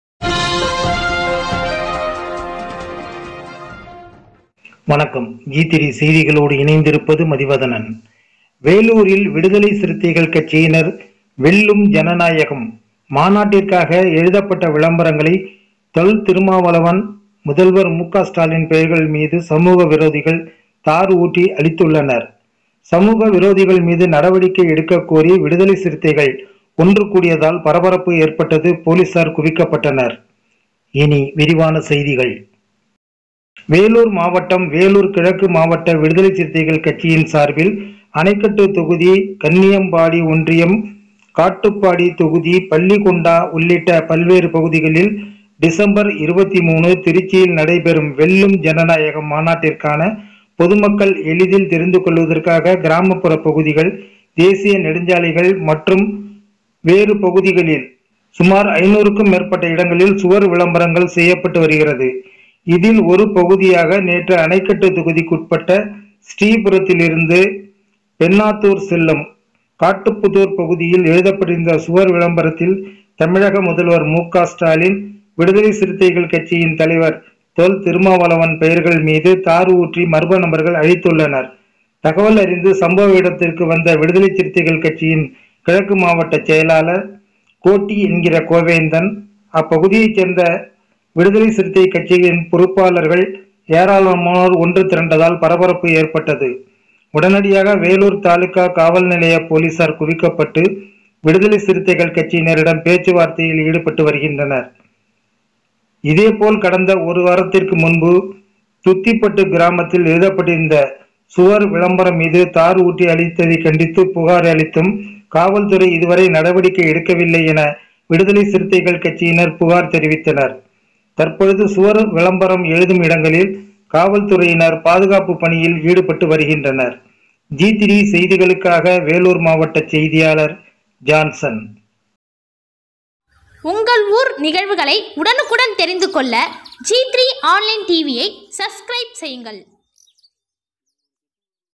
விடுதலை சிறுத்தைகள் கட்சி மாநாட்டு விளம்பரத்தின் மீது தார் ஊற்றி அழித்ததால் பரபரப்பு.
திருச்சியில் நடைபெறவிருக்கும் விடுதலை சிறுத்தைகள் கட்சியின் மாநாட்டு விளம்பரம் வேலூர் மாவட்ட பகுதிகளில் எழுதப்பட்டு இருந்தது. முதல்வர் மு க ஸ்டாலின்.
தொல்.திருமாவளவன். பெயர்கள் மீது சமூக விரோதிகள் தார்ஊற்றி அழித்ததால். அதிக அளவில் காவல்துறையினர் குவிக்கப்பட்டனர்.